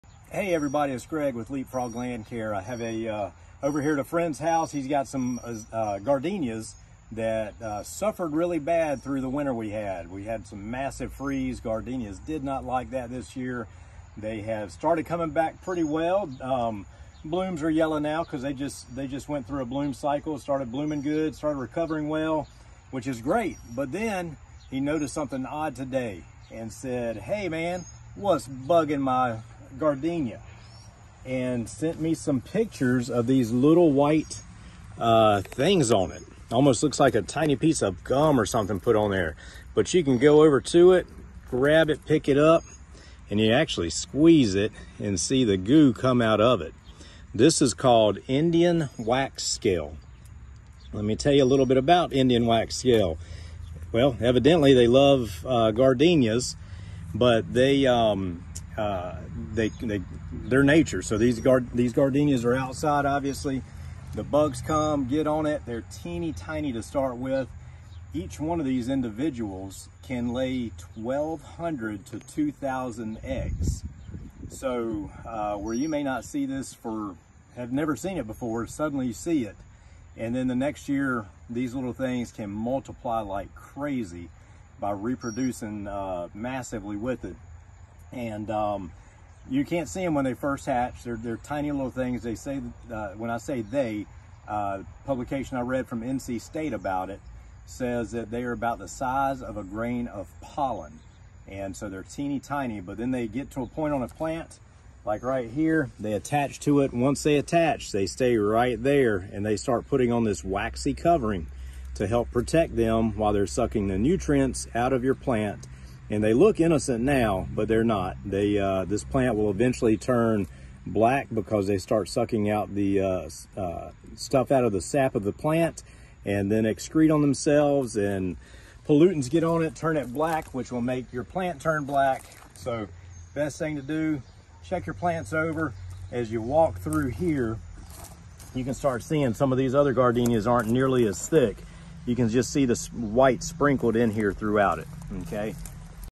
What's buggin' my shrubs? Indian Wax Scale Edition
What the heck is buggin' my shrubs!? Let's chat Indian Wax Scale with Greg and see what can be done for your shrubs when the pests come out to play!